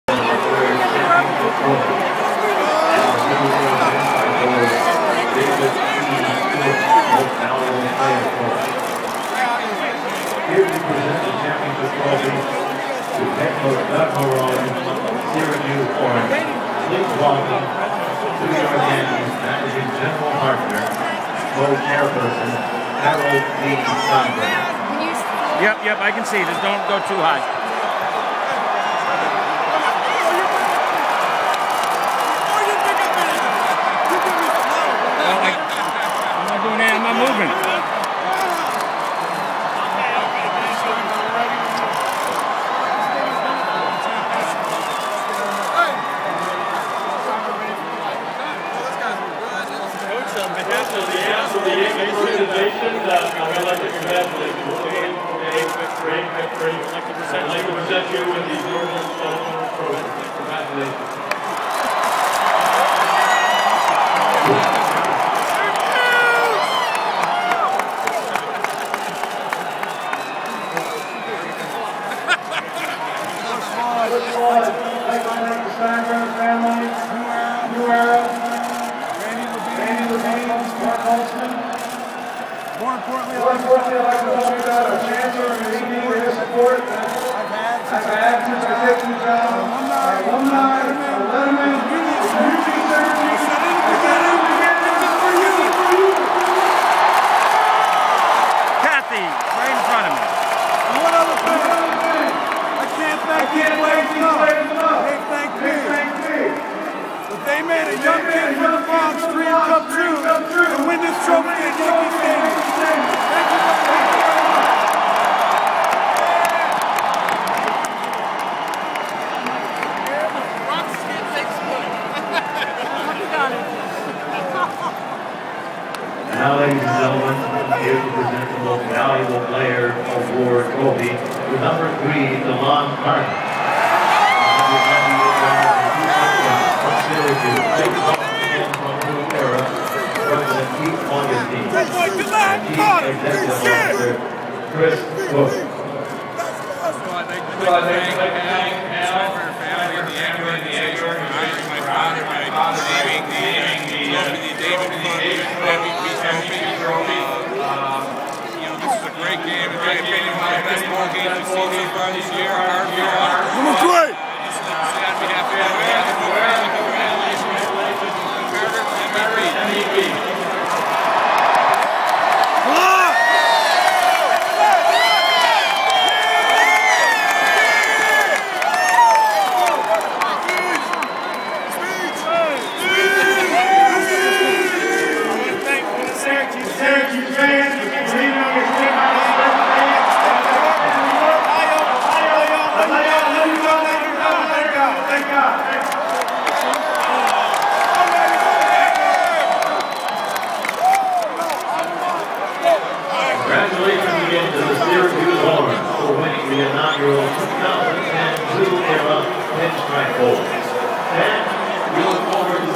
Pinstripe Bowl Trophy Presentation
The SU fan's front row seat to the Pinstripe Bowl trophy presentation.